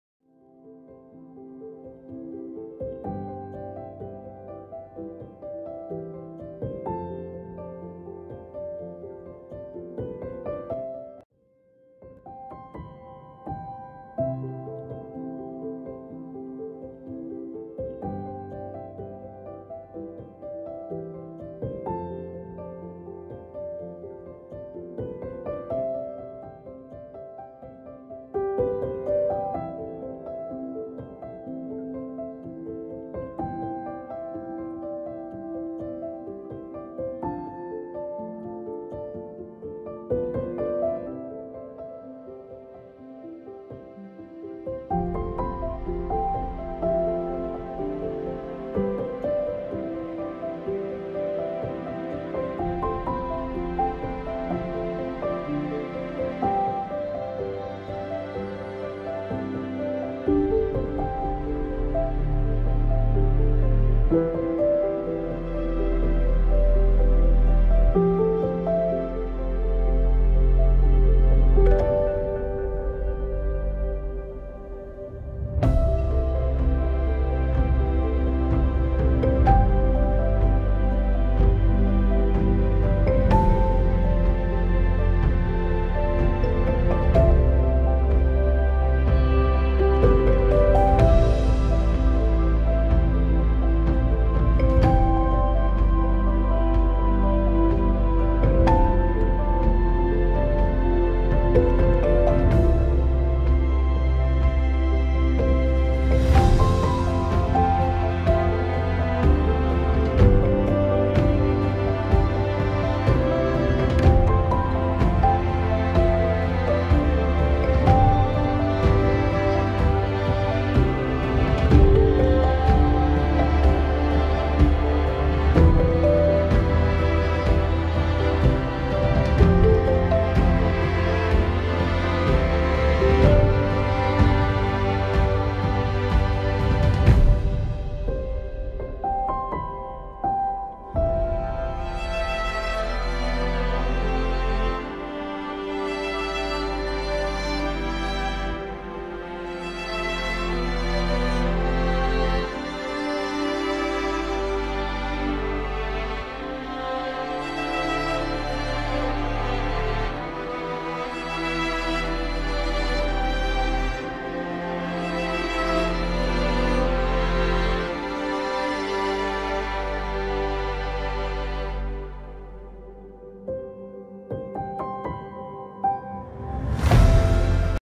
Juliana B Whisler White Dove Release Martin Funeral Home Warrenton Missouri
Beautiful Tribute Farewell Trinity Dove Release with a Flight of Angel's